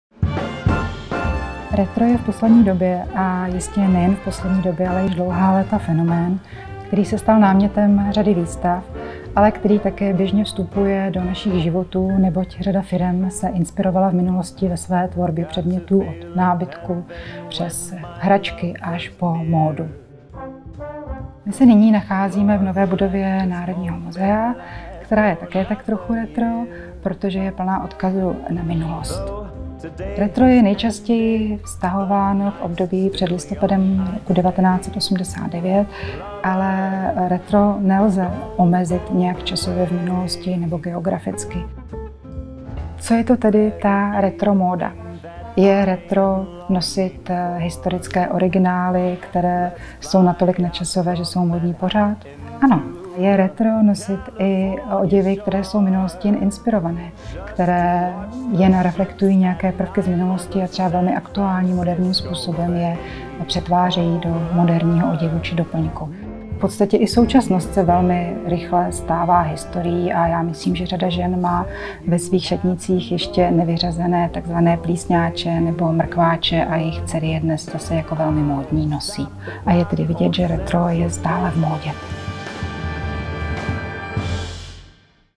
Retro
Výstavou provádí Miroslava Burianová. Navštivte nejen tuto virtuální výstavu v Národním muzeu